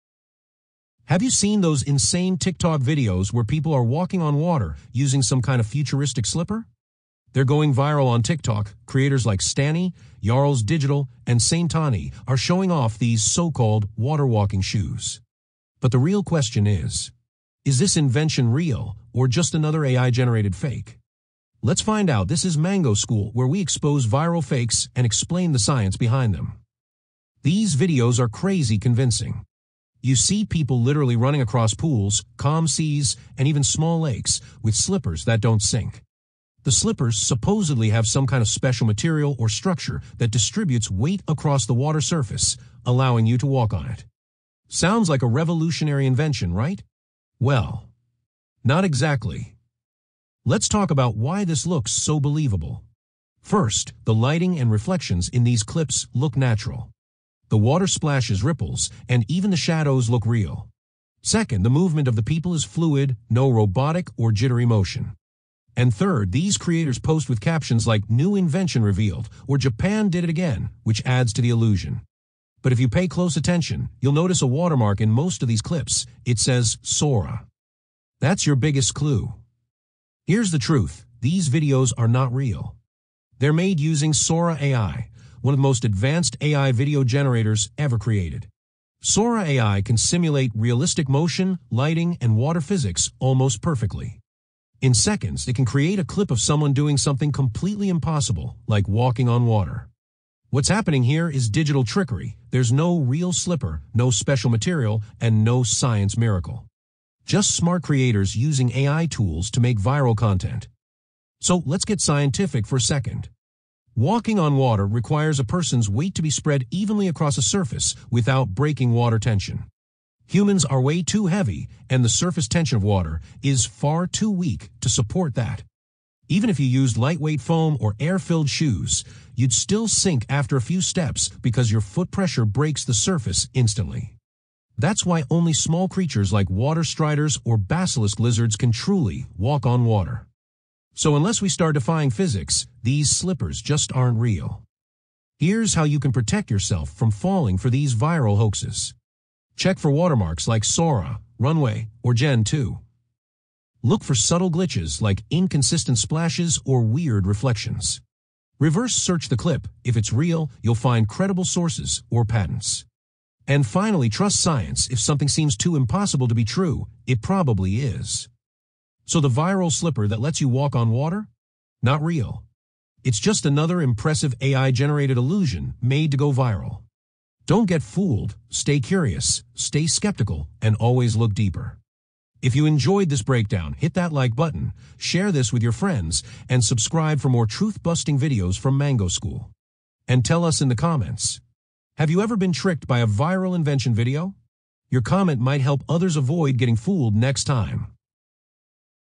Slipper That Lets You Walk on Water?! 🌊 Water Jet Shoes Real Floating Shoes Invention or AI Fake?
A viral TikTok trend claims a new slipper lets people walk on water — yes, literally! 👣💦
Clips from creators like Stantti, Jarles_Digital, and Saintantti show people running across pools and oceans using so-called “water-walking slippers.” But is it real or fake?

In this episode of Mango School, we break down the viral “walking on water” videos and expose the truth — most of these clips are AI-generated using Sora AI! 🧠💻

We’ll explain why walking on water is physically impossible, how AI can make fake inventions look 100% real, and what signs you can use to spot these digital hoaxes.